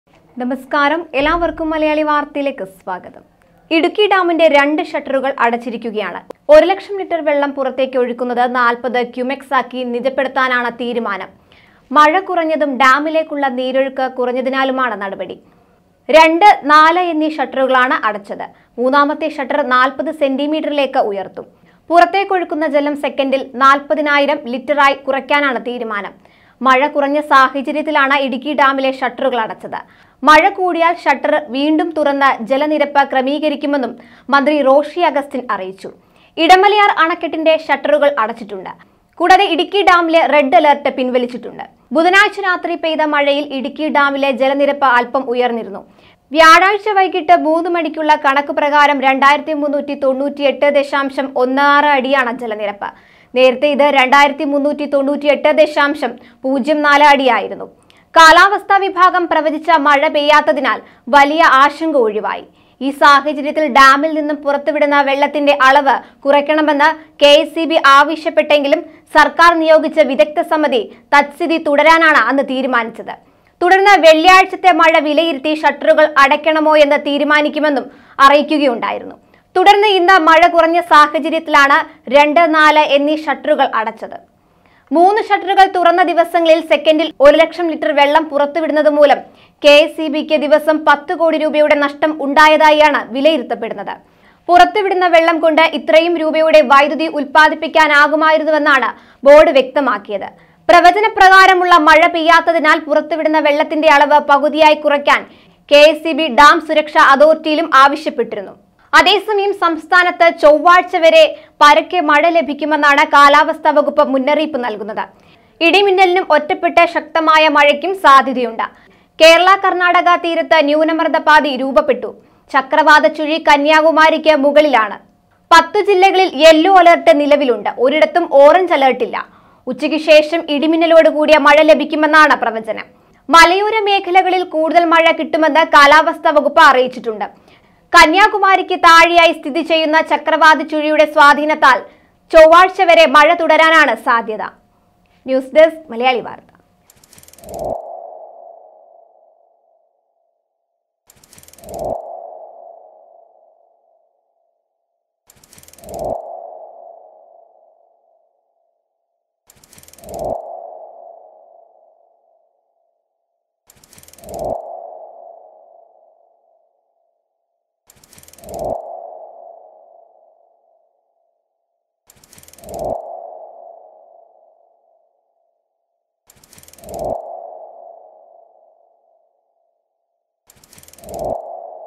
Idukkidam- ഷട്ടറുകള്‍ അടയ്ക്കുന്ന വീഡിയോ
"മൂന്നാമത്തെ ഷട്ടർ 40 സെന്റി മീറ്ററിലേക്ക് ഉയർത്തും.പുറത്തേക്ക് ഒഴുകുന്ന ജലം സെക്കന്റിൽ 40,000 ലിറ്റർ ആയി കുറക്കാനാണ് തീരുമാനം.